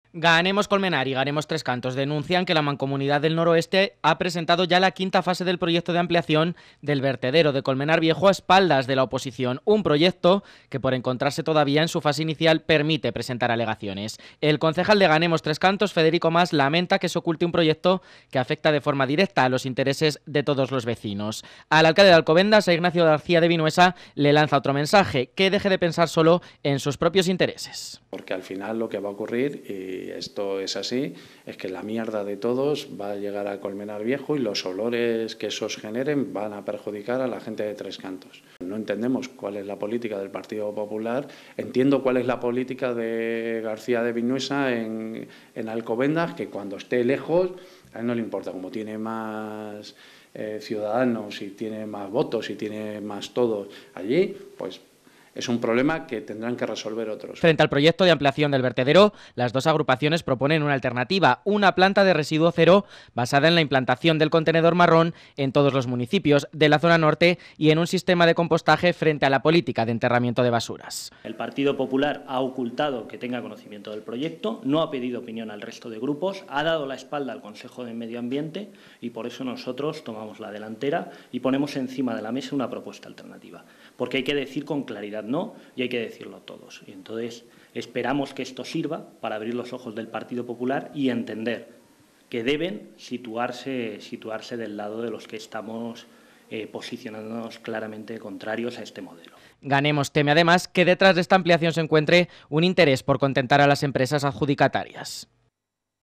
Ganemos denuncia que el PP quiere ampliar el vertedero de Colmenar "a espaldas de la oposición"
La agrupación teme que la aprobación de la quinta fase suponga "la prolongación de su vida útil" en perjuicio de los vecinos y del medio ambiente